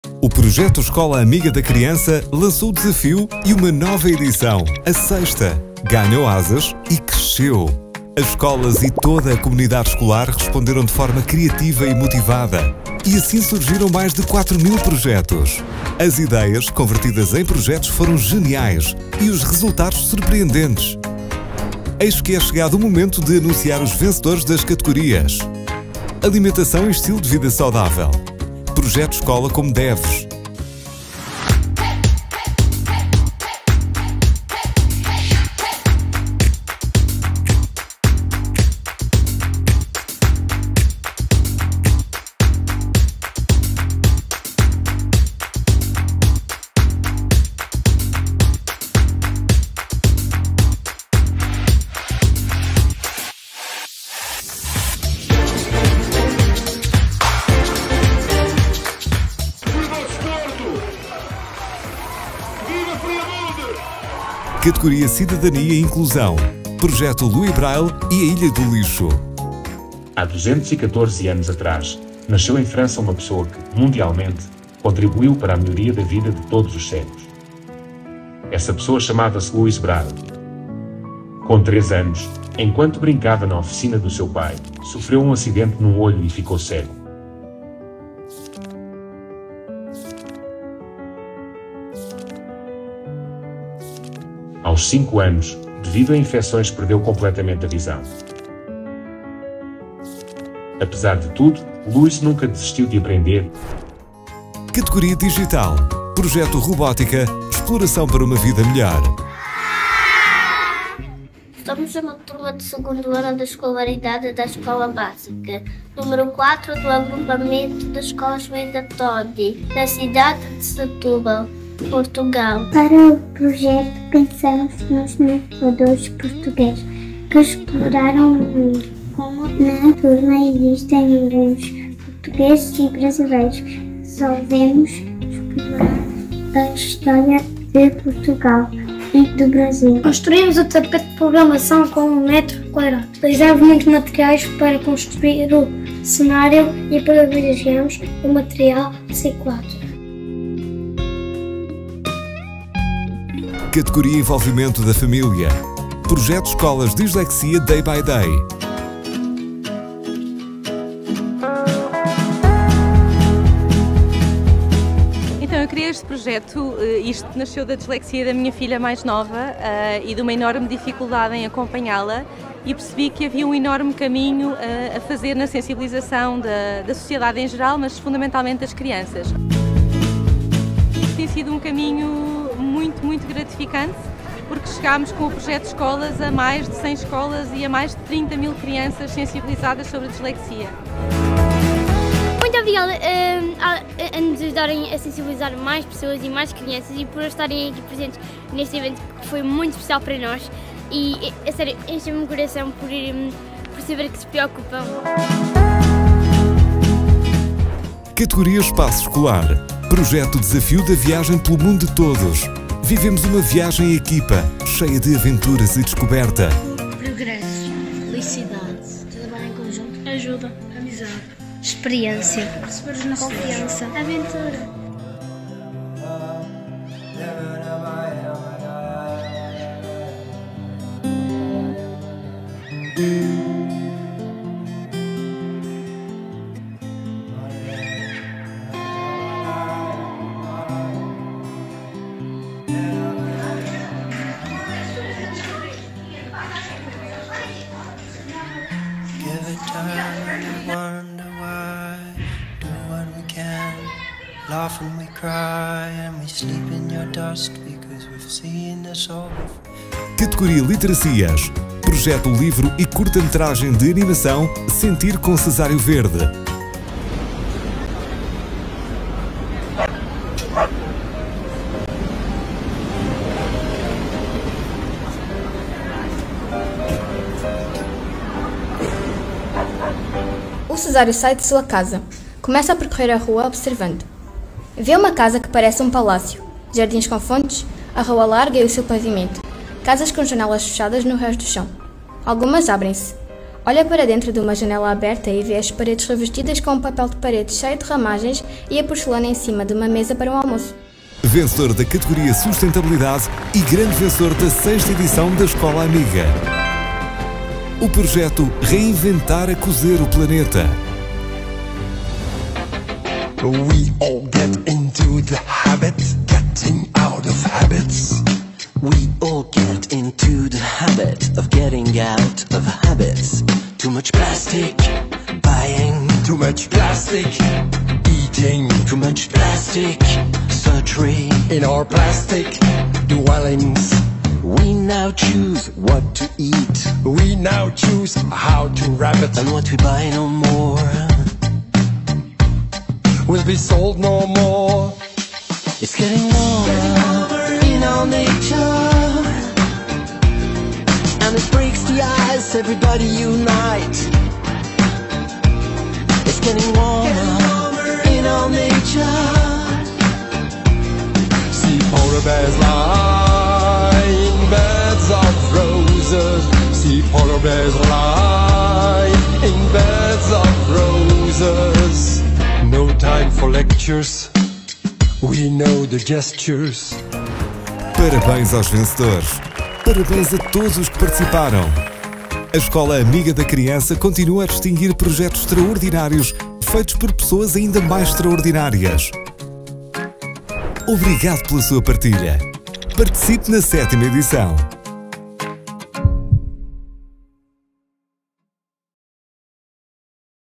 Vencedores Escola Amiga 6.ª edição
O projeto Escola Amiga da Criança lançou o desafio e uma nova edição, a 6.ª, ganhou asas e cresceu.

As escolas e toda a comunidade escolar responderam de forma criativa e motivada e assim surgiram mais de 4000 projetos!

As ideias convertidas em projetos foram geniais e os resultados surpreendentes!

Eis que é chegado o momento de anunciar os vencedores das categorias:

Alimentação e estilo de vida saudável: Projeto Escola como DEV(s).

Cidadania e inclusão: Projeto Louis Braille e a ilha do Lixo.

Digital: Projeto Robótica – Exploração para uma vida melhor.

Envolvimento da Família: Projeto Escolas Dislexia Day by Day.

Espaço escolar: Projeto Desafio da viagem pelo mundo de todos. Vivemos uma viagem em equipa, cheia de aventuras e descoberta.

Literacias: Projeto Livro e Curta-Metragem de Animação “Sentir com Cesário Verde”.

Vencedor da categoria Sustentabilidade e grande vencedor da 6.ª edição da Escola Amiga: o projeto Reinventar a Coser o Planeta!

Parabéns aos vencedores. Parabéns a todos os que participaram.
A Escola Amiga da Criança continua a distinguir projetos extraordinários feitos por pessoas ainda mais extraordinárias.
Obrigado pela sua partilha!
Participe na 7.ª edição!

©Ilustrações do projeto Dislexia Day by Day da autoria de Tiago Albuquerque